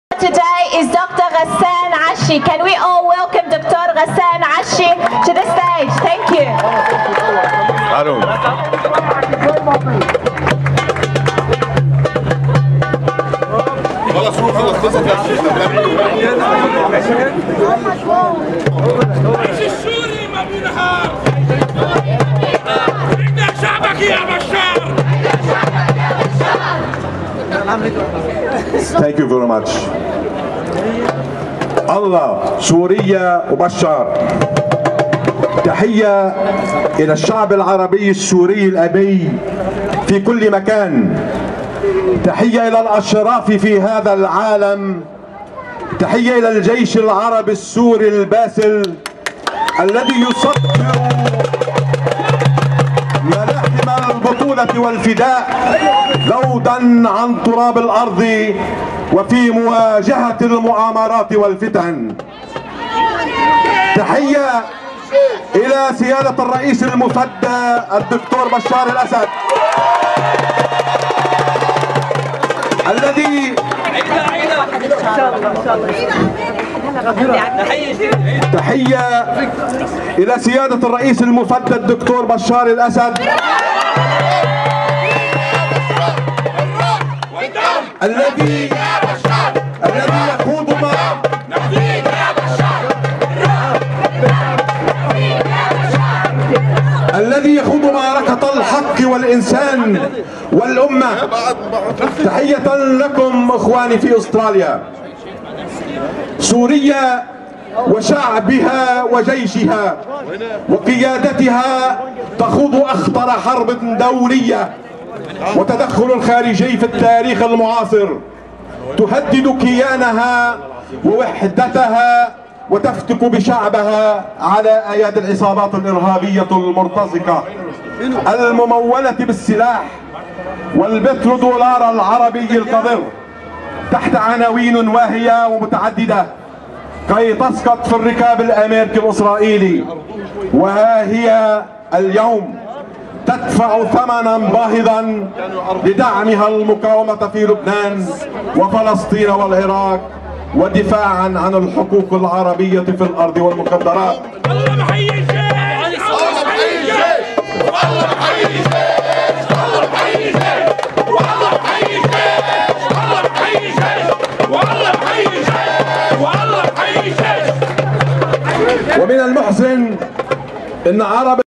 Hands off Syria rally, Sydney 5 August 2012, Dr Ghassan Achi
Part of speech by Dr Ghassan Achi, Town Hall Square